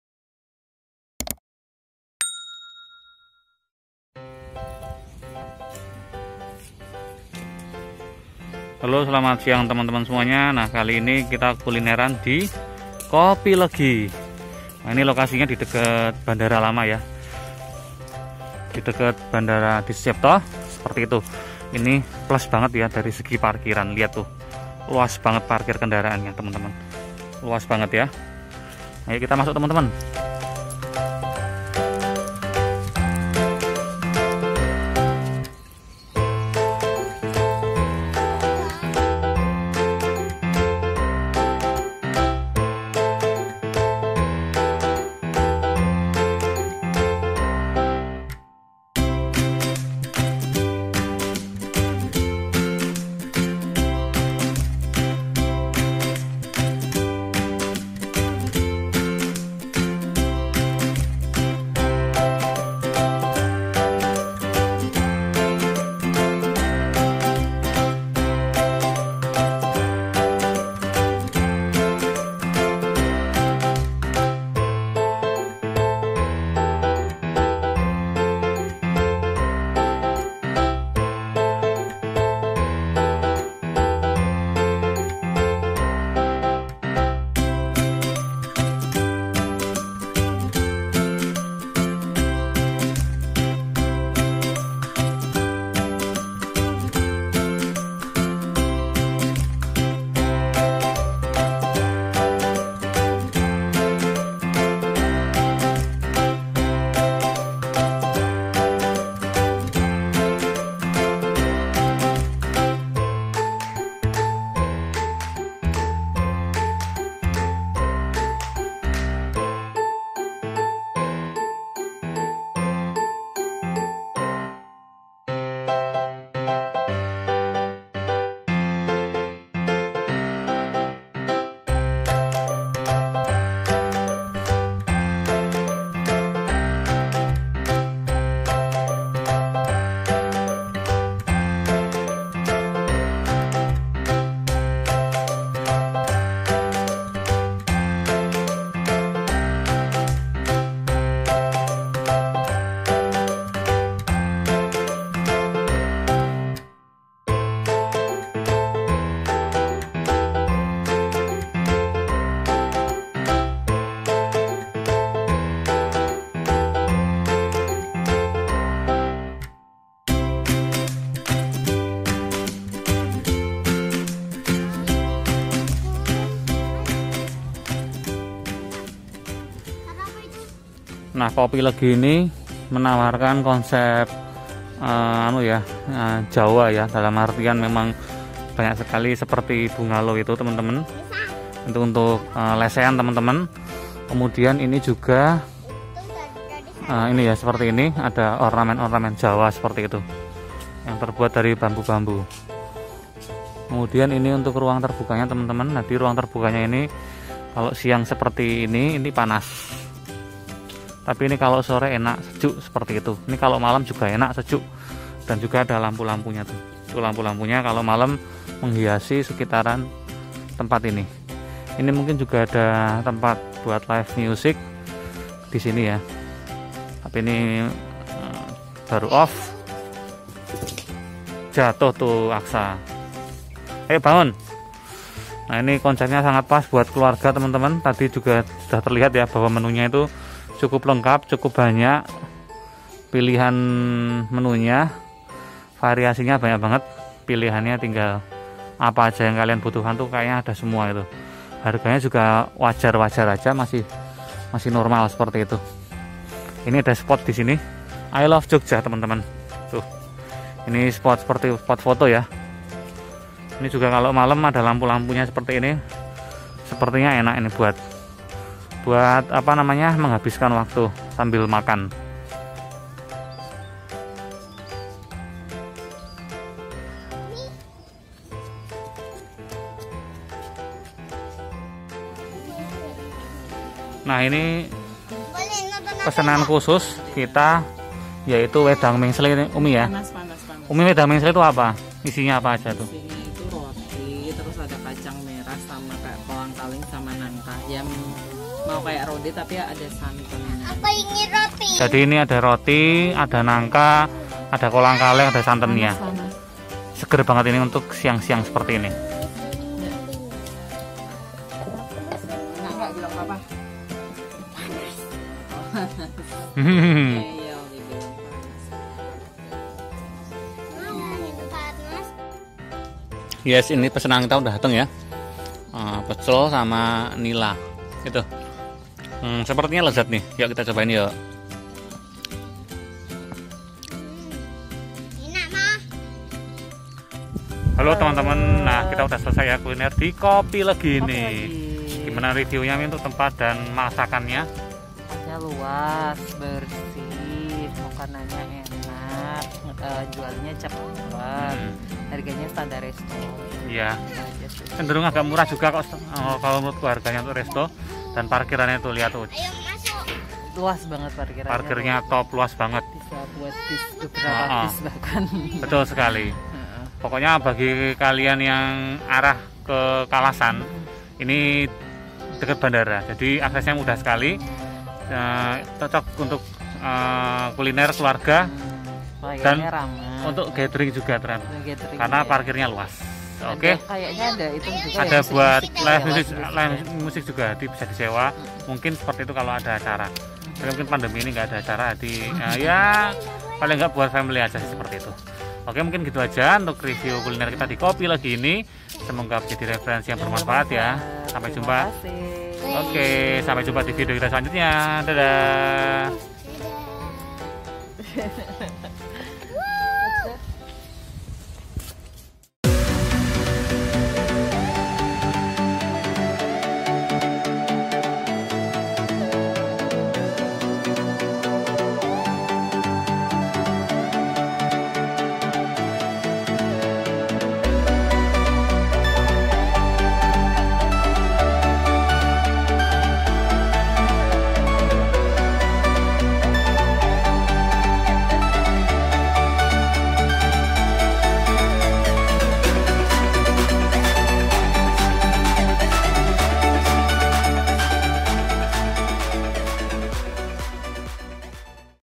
KULINER JOGJA - RESTO ASRI DENGAN TAMAN YANG INDAH - KOPI LEGI, SLEMAN
THE SASMITA'S DIARY - KULINERAN

Referensi dan Review Kuliner di Jogja ala ala IRFA HUDAYA SASMITA & Keluarga

"Kopi Legi, Sleman, Daerah Istimewa Yogyakarta"
Detail Alamat = Cek GOOGLE MAPS Yaaa…

Follow Instagram / Facebook Kami Yaaa…
***@kulinerjogjagaul / Kuliner Jogja Paling GAUL!

Skor Versi Kita (1-10)
-Rasa = 9
-Variasi Menu = 9
-Harga = 8
-Pelayanan = 9
-Fasilitas Penunjang = 8
-Parkir Mobil = 10